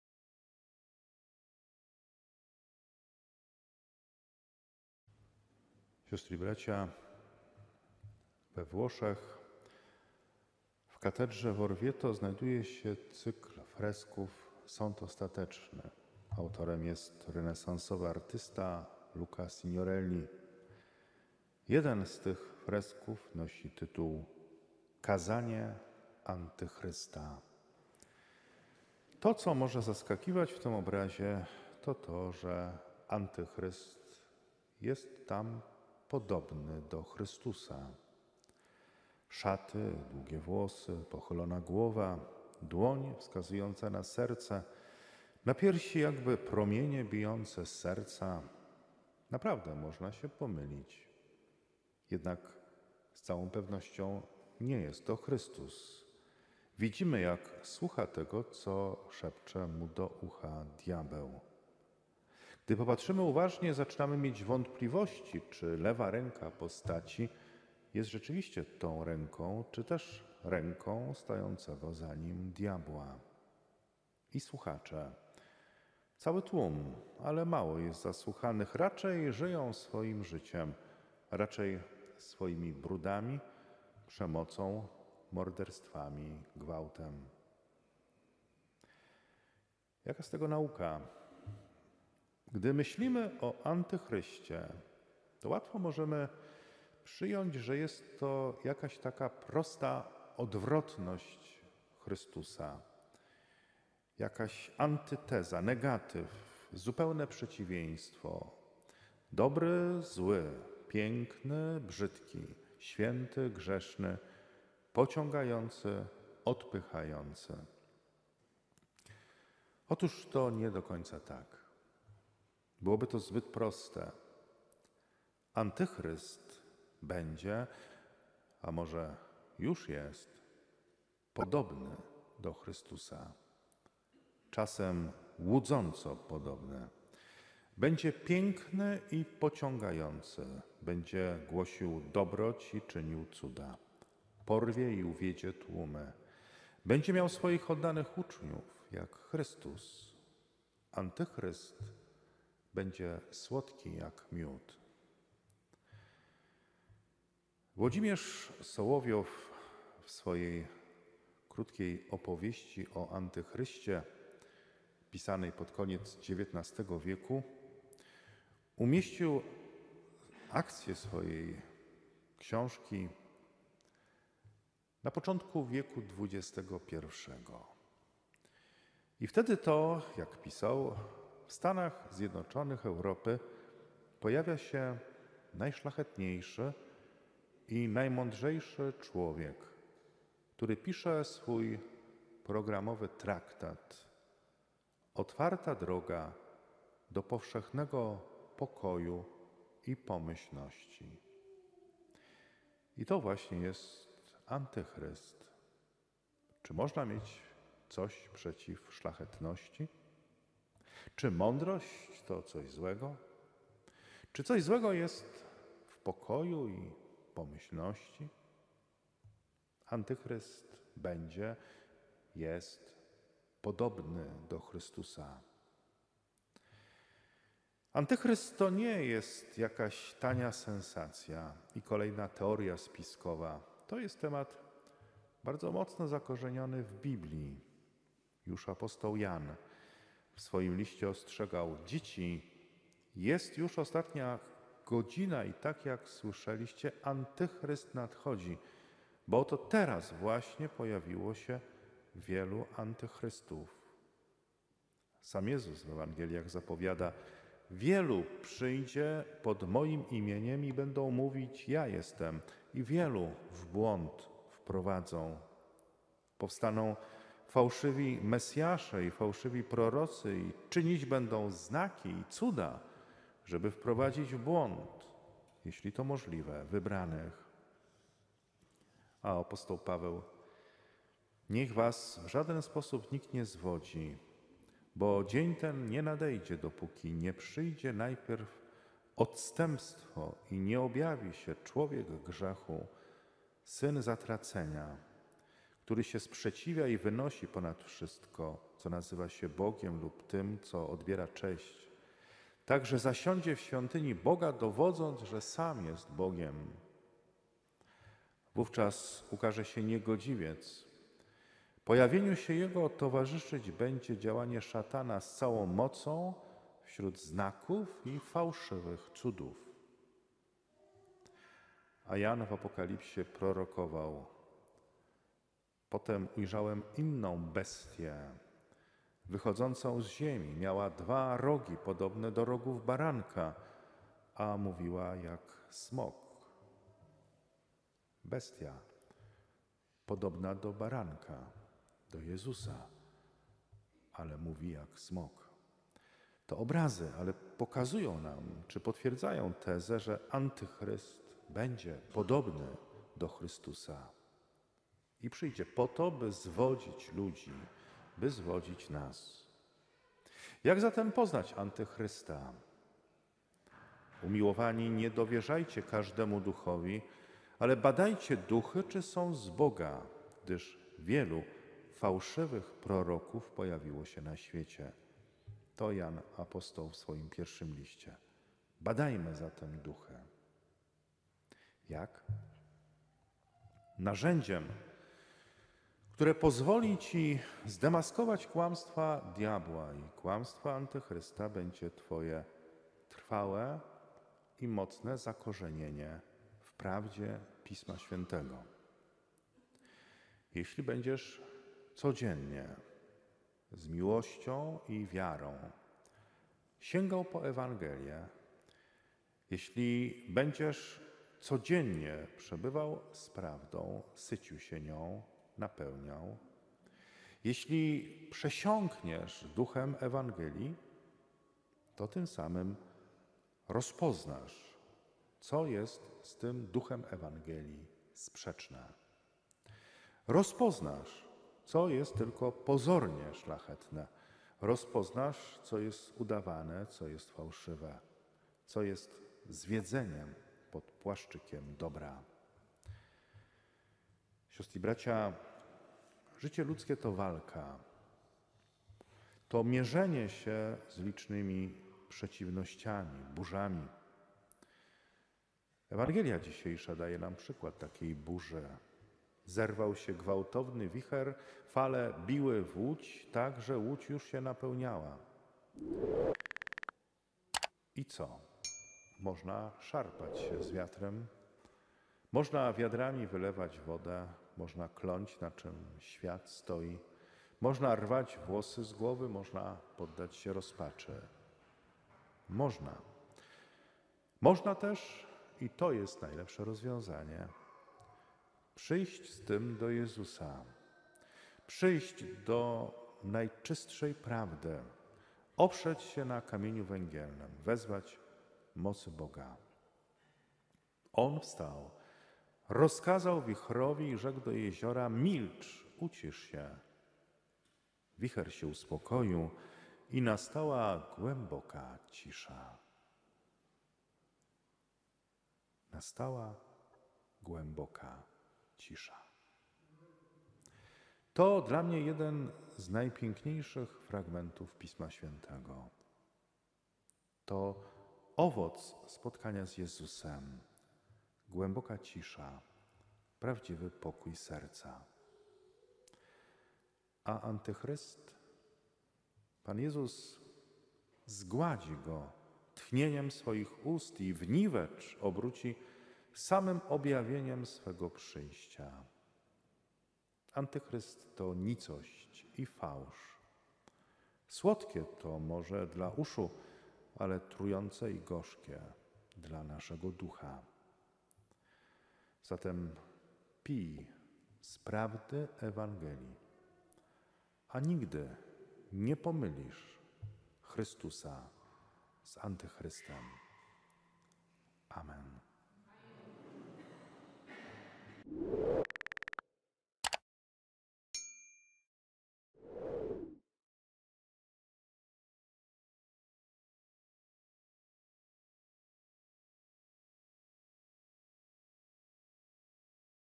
🔴 Kazanie antychrysta
12. Niedziela Zwykła, B; (Rdz 3, 9-15); (Ps 130 (129), 1b-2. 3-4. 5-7a. 7b-8); (2 Kor 4, 13 – 5, 1); Aklamacja (J 12, 31b-32); (Mk 3, 20-35); Winnica 6 czerwca 2021.

Fundacja Nasza Winnica
Od Litery Do Wiary
Rzeczy Dobrze Pomyślane